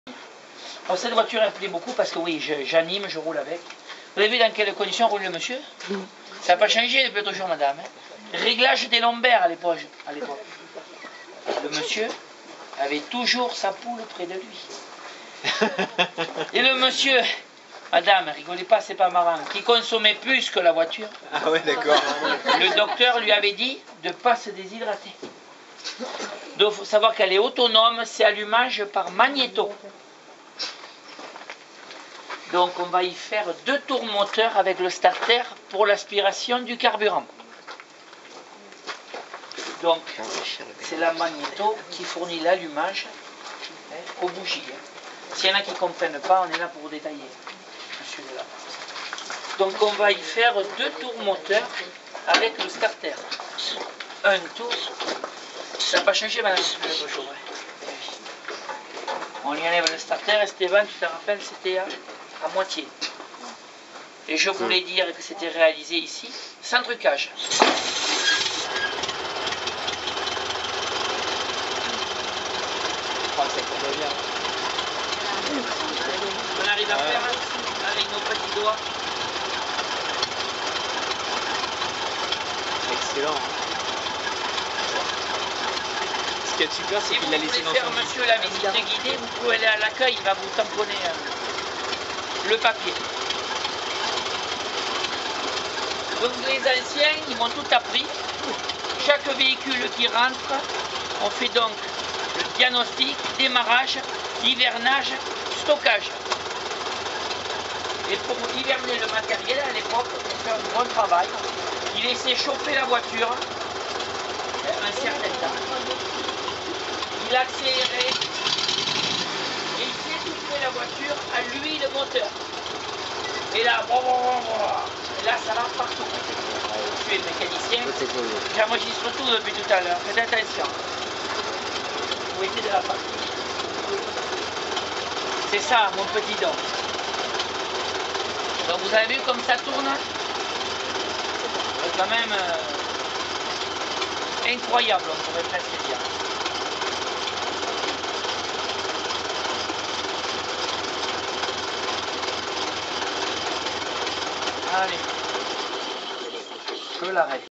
Voiture Citroën type A
c'est une voiture qui était la concurrente de la Peugeot quadriette.